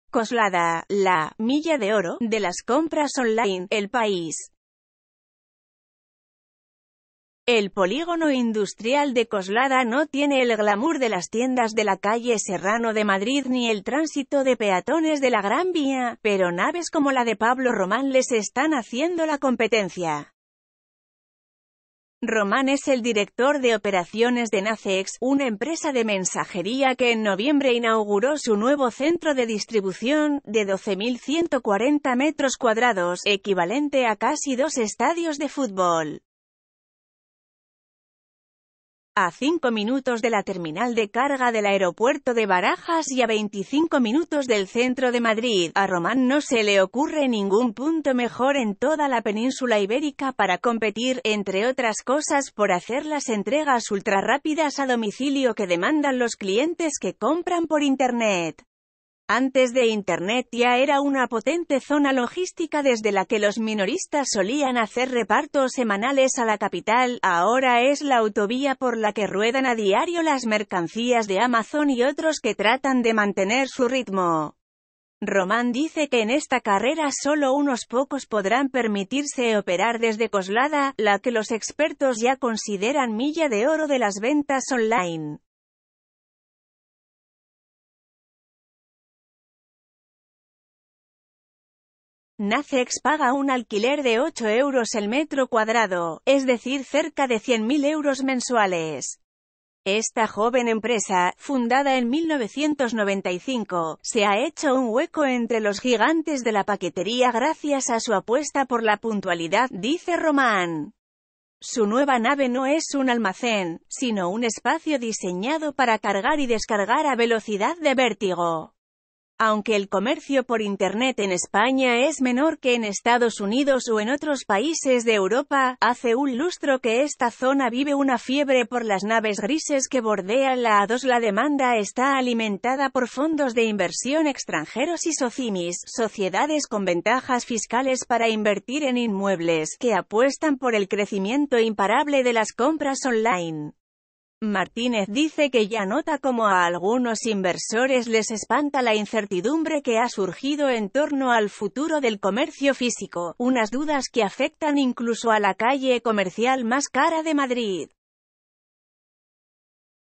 coslada, la ‘milla de oro’ de las compras online (elpais)
El polígono industrial de coslada no tiene el glamour de las tiendas de la calle serrano de madrid ni el tránsito de peatones de la gran vía, pero naves como la de pablo román les están haciendo la competencia. Román es el director de operaciones de nacex, una empresa de mensajería que en noviembre inauguró su nuevo centro de distribución, de 12140 metros cuadrados (equivalente a casi dos estadios de fútbol). A cinco minutos de la terminal de carga del aeropuerto de barajas y a 25 minutos del centro de madrid, a román no se le ocurre ningún punto mejor en toda la península ibérica para competir, entre otras cosas, por hacer las entregas ultrarrápidas a domicilio que demandan los clientes que compran por internet. Antes de internet ya era una potente zona logística desde la que los minoristas solían hacer repartos semanales a la capital, ahora es la autovía por la que ruedan a diario las mercancías de amazon y otros que tratan de mantener su ritmo. Román dice que en esta carrera solo unos pocos podrán permitirse operar desde coslada, la que los expertos ya consideran milla de oro de las ventas online. Nacex paga un alquiler de ocho euros el metro cuadrado, es decir cerca de 100000 euros mensuales. Esta joven empresa, fundada en 1995, se ha hecho un hueco entre los gigantes de la paquetería gracias a su apuesta por la puntualidad, dice román. Su nueva nave no es un almacén, sino un espacio diseñado para cargar y descargar a velocidad de vértigo. Aunque el comercio por internet en españa es menor que en estados unidos o en otros países de europa, hace un lustro que esta zona vive una fiebre por las naves grises que bordean la a-2 la demanda está alimentada por fondos de inversión extranjeros y socimis (sociedades con ventajas fiscales para invertir en inmuebles) que apuestan por el crecimiento imparable de las compras online. Martínez dice que ya nota cómo a algunos inversores les espanta la incertidumbre que ha surgido en torno al futuro del comercio físico, unas dudas que afectan incluso a la calle comercial más cara de madrid.